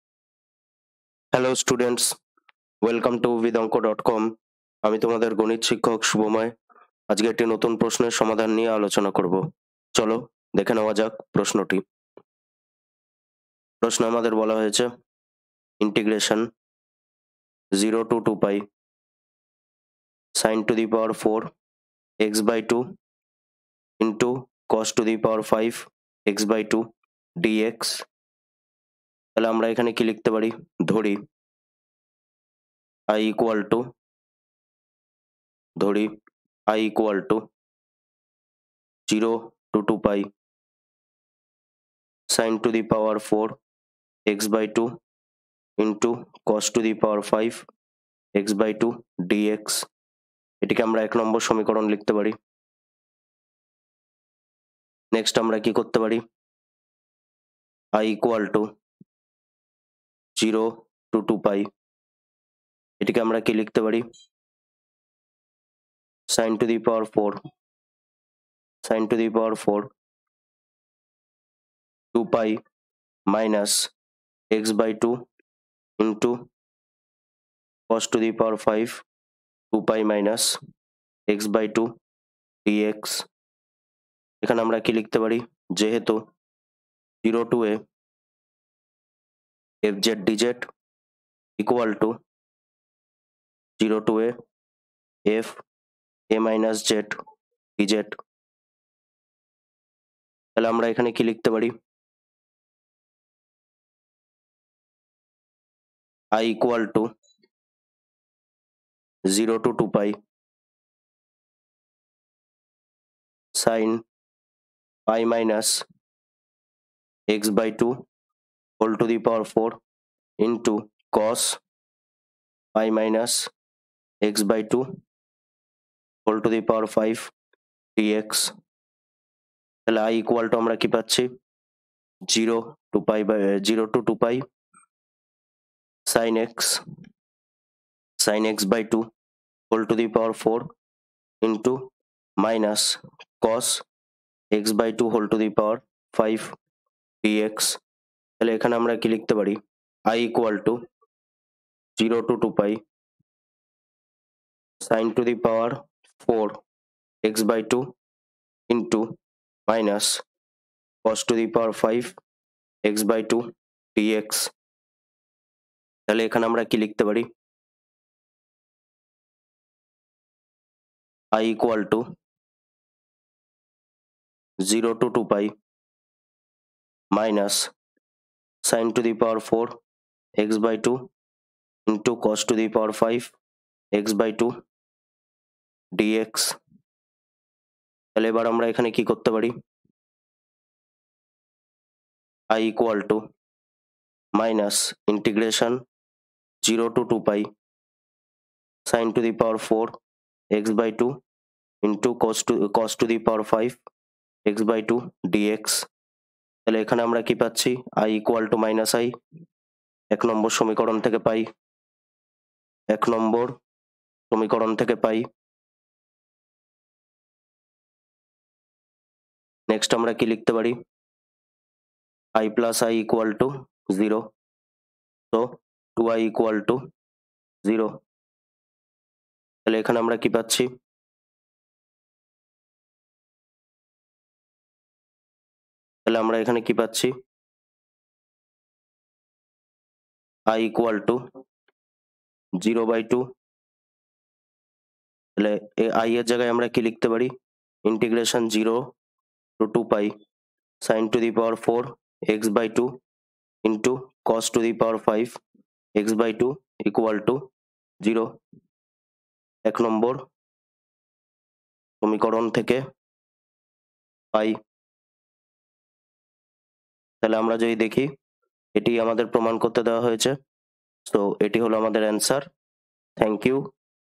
West Bengal Board Class 12 Math Book Solution in Bengali - S N Dey Exercise Question: 16B.2.15
Get free Maths book video and PDF solutions in Bengali for S N Dey. Free answer and explanation through video for question 16B.2.15, of Chapter Definite Integral from S N Dey followed for Class 12, West Bengal Council of Higher Secondary Education (WBCHSE).